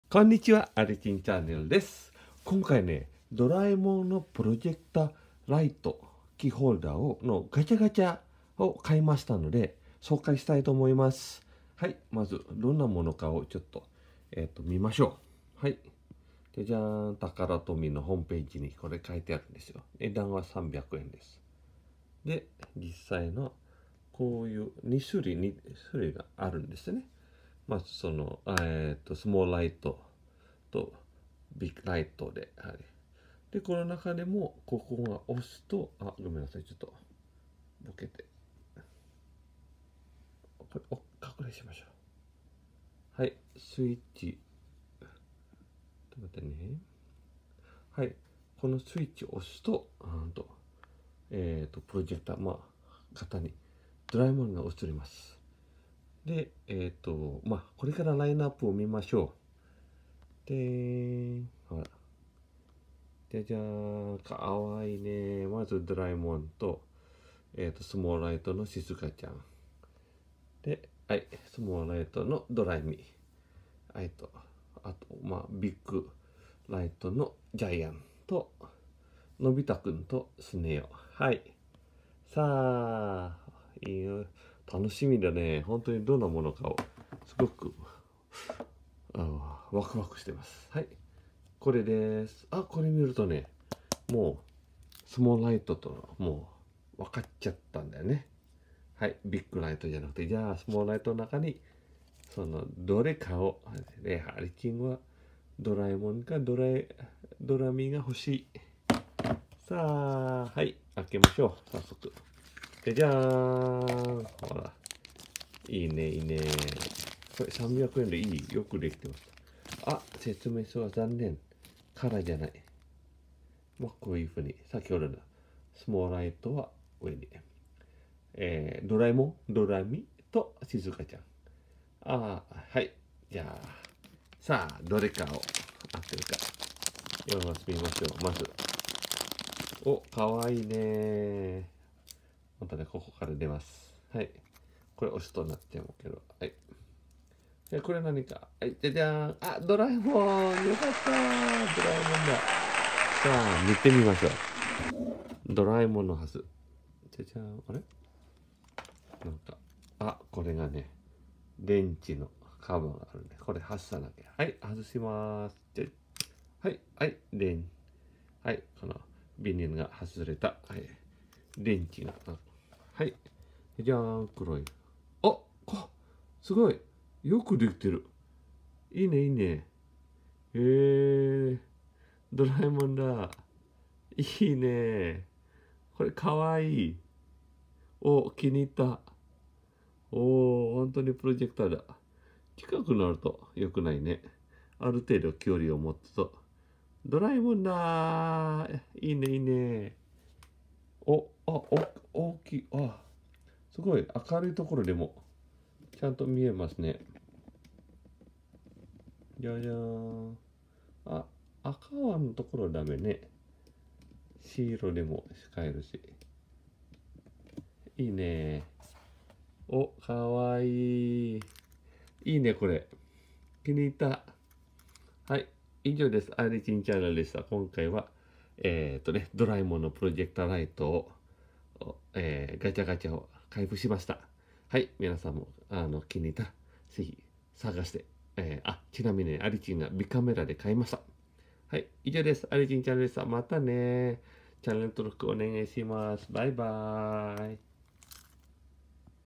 ドラえもんのプロジェクターライトキーホルダーの紹介 Let’s open Doraemon Projector Light Gacha
今回は、ドラえもんのプロジェクターライトキーホルダーのガチャガチャを開封してレビューします🤩
このガチャガチャはいいよ😍
是非動画を見てね🎶